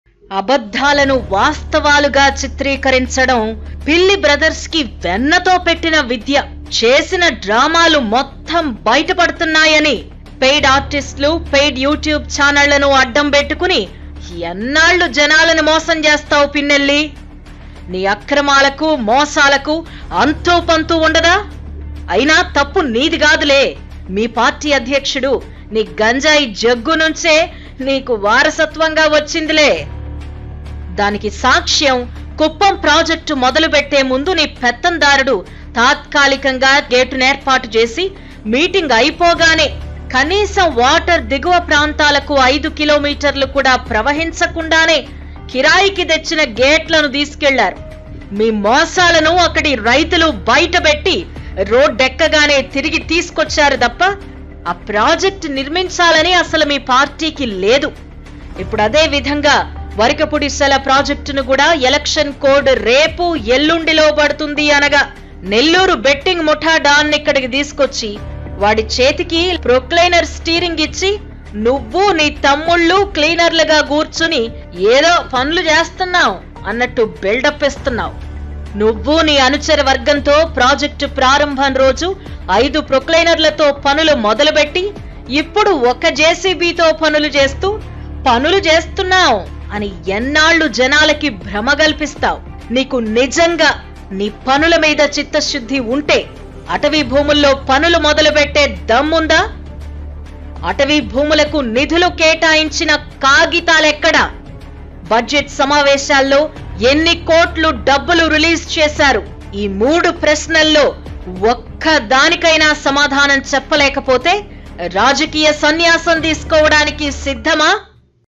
అబద్ధాలను వాస్తవాలుగా చిత్రీకరించడంలో పిల్లి బ్రదర్స్ కి వెన్నతో పెట్టిన విద్య.. | Macherla
అబద్ధాలను వాస్తవాలుగా చిత్రీకరించడంలో పిల్లి బ్రదర్స్ కి వెన్నతో పెట్టిన విద్య..
చేసిన డ్రామాలు మొత్తం బయట పడుతున్నాయి అని పైడ్ ఆర్టిస్టులు, పెయిడ్ యూట్యూబ్ ఛానెళ్లను అడ్డం  పెట్టుకొని  ఎన్నాళ్లు జనాలని మోసం చేస్తావు పిన్నెల్లి..
నీ అక్రమాలకు,మోసాలకు అంతు,పంతు ఉండదా...
అయిన తప్పు నీది కాదులే మీ పార్టీ అధ్యక్షుడు నీ గంజాయి జగ్గు నుంచే నీకు వారసత్వంగా వచ్చింది లే..
దానికి సాక్ష్యం కుప్పం ప్రాజెక్టు మొదలు పెట్టే ముందు నీ పెత్తందారుడు తాత్కాలికంగా  చెక్క గేటును ఏర్పాటు చేసి మీటింగ్ అయిపోగానే కనీసం వాటర్ దిగువ ప్రాంతాలకు 5 కిలోమీటర్లు కూడా ప్రవహించకుండనే కిరాయికి  తెచ్చిన గేట్లను తీసుకెళ్లారు. మీ  మోసాలను అక్కడ రైతులు బయటపెట్టి రోడ్డు ఎక్కారు. ఆ ప్రాజెక్ట్ నిర్మించాలని అసలు మీ పార్టీకి లేదు..
ఇప్పుడు అదే విధంగా వరికపుడిసెల ప్రాజెక్ట్ ను కూడా  ఎలక్షన్ కోడ్ రేపు ఎల్లుండిలో పడుతుంది అనగా నెల్లూరు బెట్టింగ్ ముఠా డాన్ ని ఇక్కడికి తీసుకొచ్చి, చేతికి ప్రొక్లైనర్ స్టీరింగ్ ఇచ్చి,నువ్వు నీ తమ్ముడు క్లీనర్లు గా కూర్చొని ఎదో పనులు చేస్తున్నాం అన్నట్టు బిల్డ్ అప్ ఇస్తున్నావ్.
నువ్వు,నీ అనుచర వర్గంతో ప్రాజెక్ట్ ప్రారంభం రోజు 5 ప్రోక్లైనర్ల తో పనులు మొదలు పెట్టీ, ఇప్పుడు ఒక దానితో పనులు చేస్తూ పనులు చేస్తున్నాం అనట్టు  ఎన్నాళ్లు  జనాలకి భ్రమ కల్పిస్తావ్...
ఈ క్రింది ప్రశ్నలకు సమాధానం చెప్పాలి..
 *1)నీకు నిజంగా నీ పనులు మీద చిత్త శుద్ధి ఉంటే  అటవీ భూముల్లో పనులు మొదలు పెట్టే దమ్ముందా..?*
*2)అటవీ భూములకు నిధులు కేటాయించిన కాగితాలు ఎక్కడ..?*
*3)బడ్జెట్ సమావేశాల్లో  ఎన్ని కోట్లు డబ్బులు రిలీజ్ చేశారు..?*
*ఈ మూడు ప్రశ్నల్లో ఒక్కదానికి అయిన సమాధానం చెప్పలేకపోతే రాజకీయ సన్యాసం తీసుకోవడానికి సిద్ధమా..?*